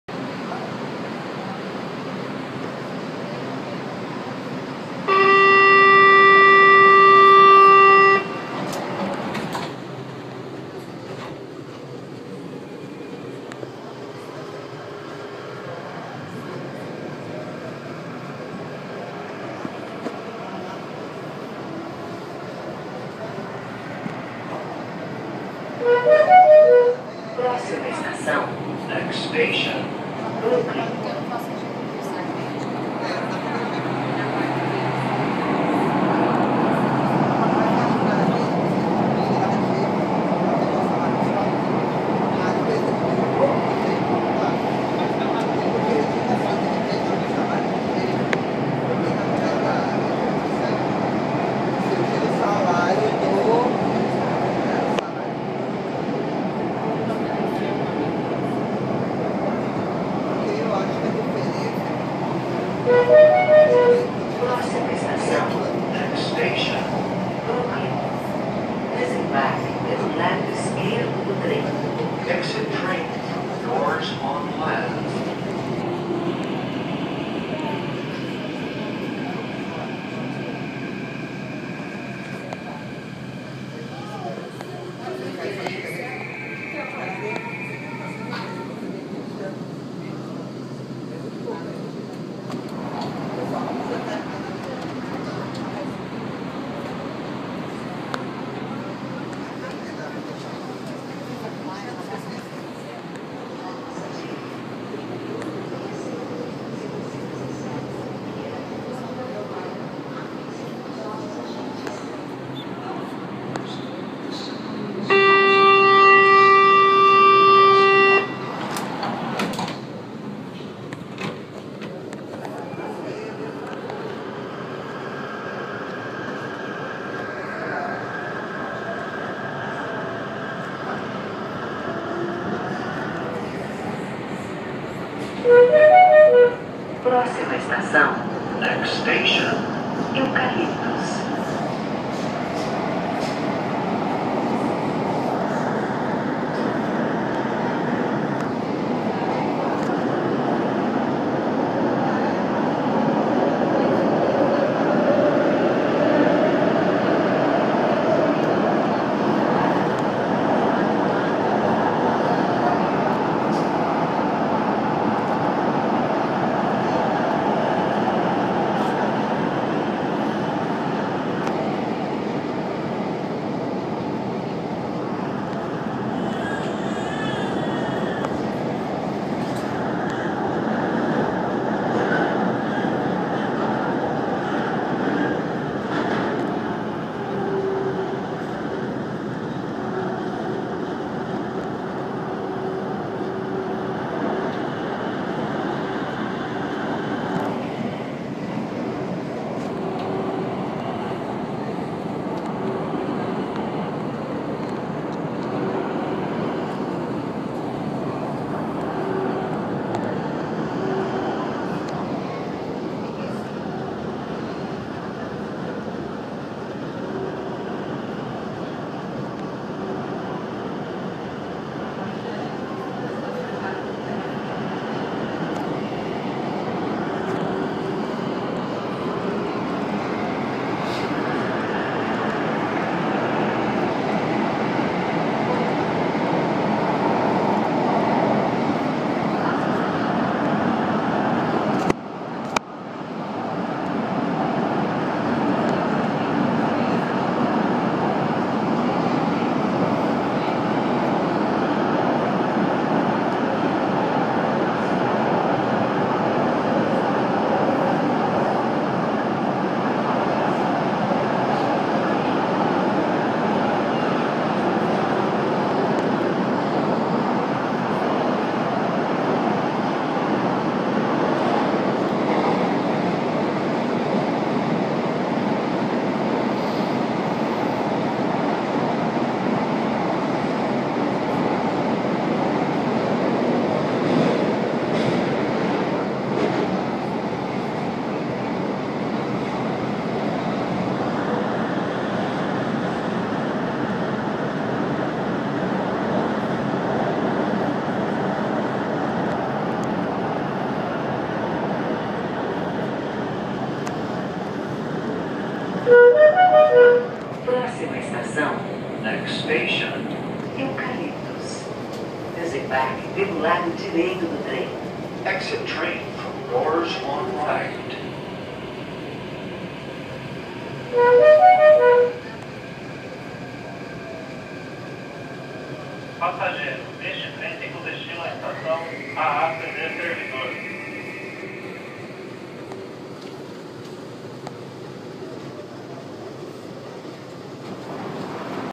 Trecho entre Borba Gato e Eucaliptos no TUE 520 - Linha 5 Lilás
Filmado no trem 520 (carro 5201)
Data: 01/09/18